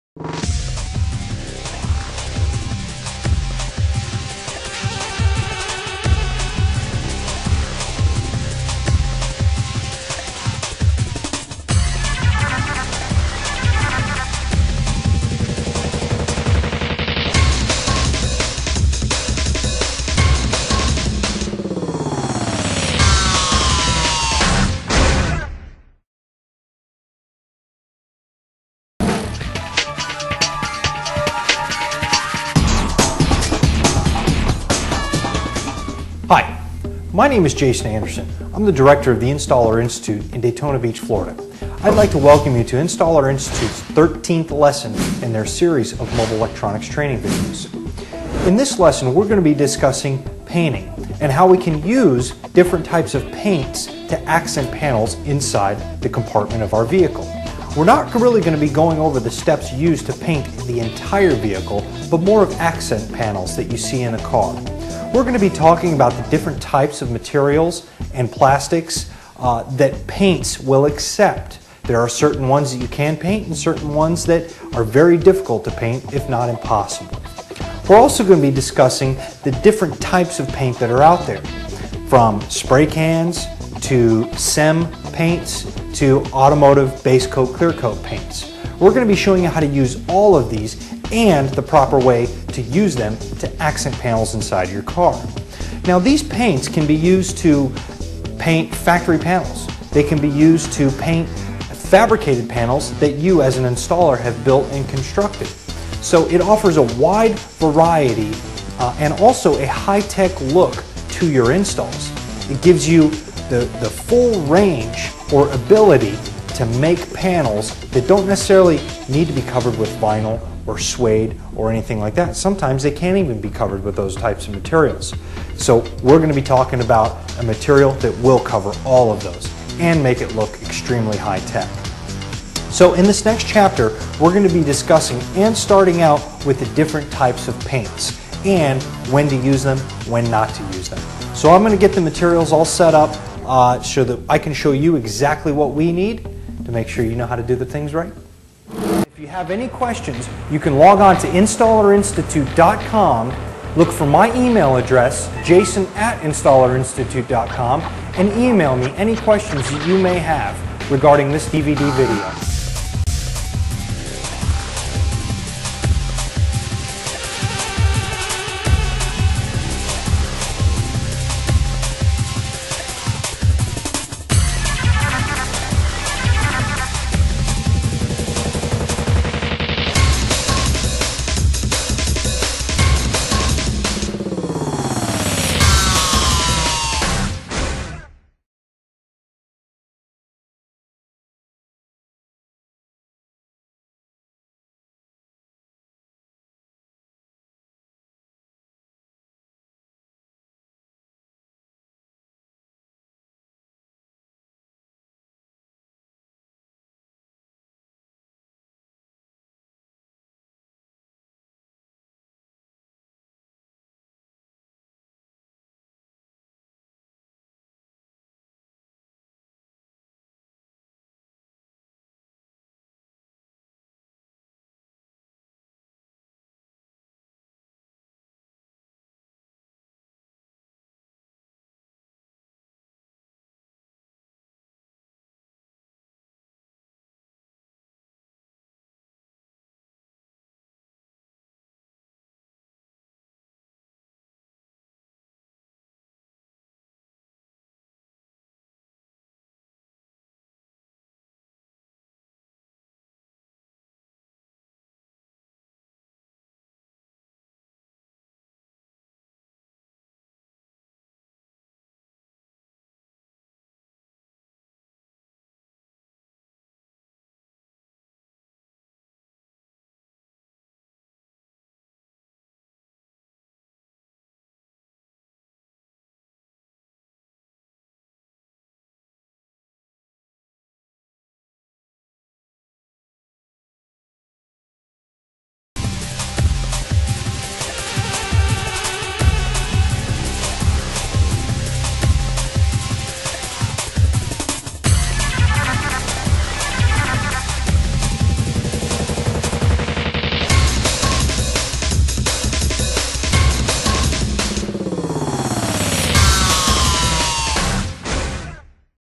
DVD 13 Painting Interior Panels Promo Video
Lesson 13  Painting Interior Panels               66 Min

In lesson 13 Painting Interior Panels, Jason will once again be teaching the proper steps for painting small fabricated projects and interior panels from the 2005 Honda Civic. This lesson will show you how to use the most common type of paint to finish those projects you put so much effort into. In this video Jason will be applying high-build primer and two stage paint to the panels that he created form the 11th and 12th lesson. Through the instruction and examples in this lesson you will be able to acquire the necessary skills to start this project and bring it to completion.

for more information.